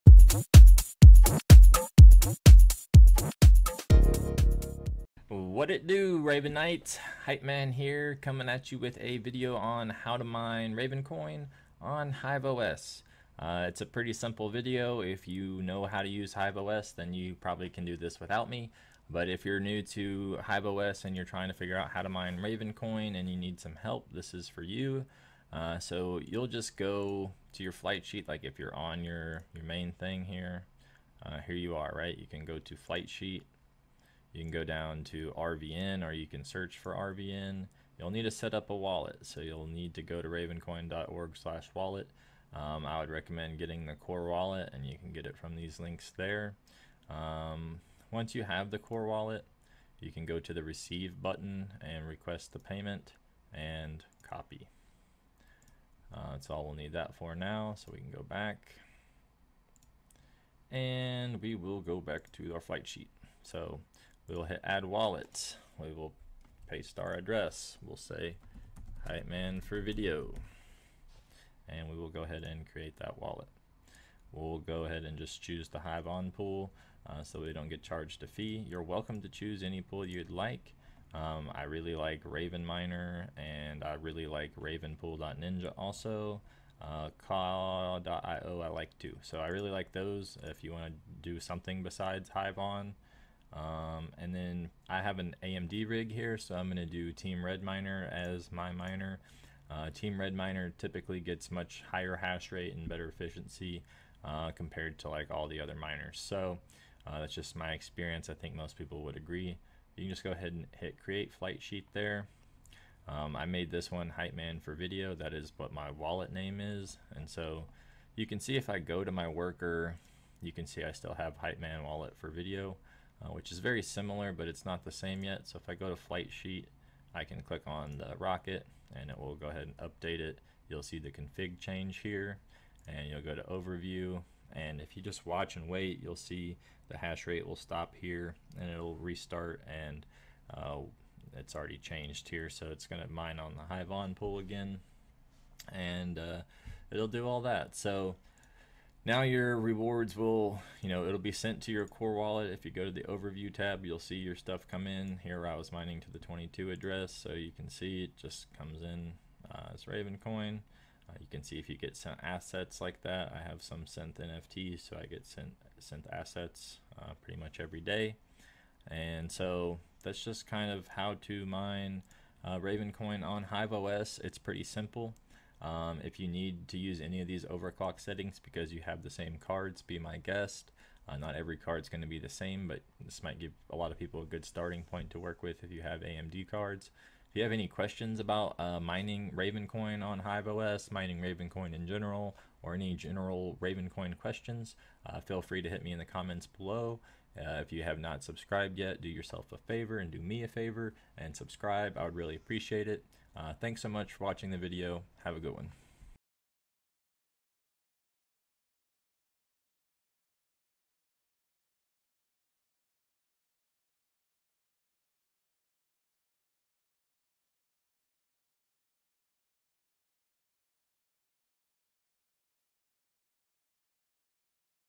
How To Mine Ravencoin on Hive OS!
In this one, we show you how to create a flight sheet in order to mine Ravencoin on Hive OS, or any other pool. Very simple setup.

When dealing with crypto currency is it important to do your own research.  Ravencoin is a peer-to-peer blockchain, handling the efficient creation and transfer of assets from one party to another.